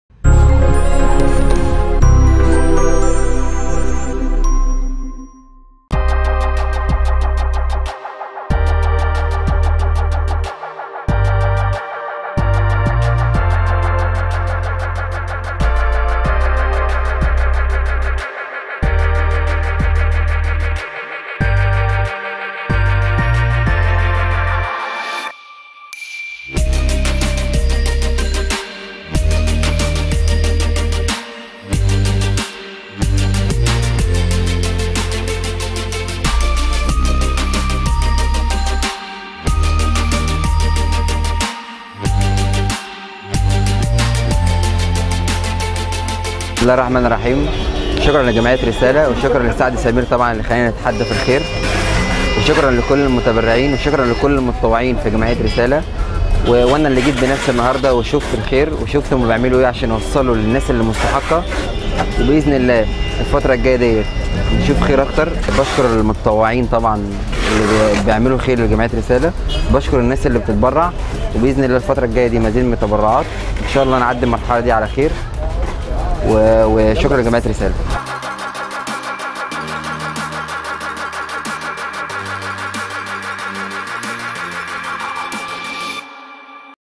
الكابتن كهربا خلال زيارته لـ تحدي الخير في جمعية رسالة : بشكر سعد سمير وكل الناس اللي اتبرعت
للدعاية والاعلان في قناة كلاكيت : klaketofficial @gmail.com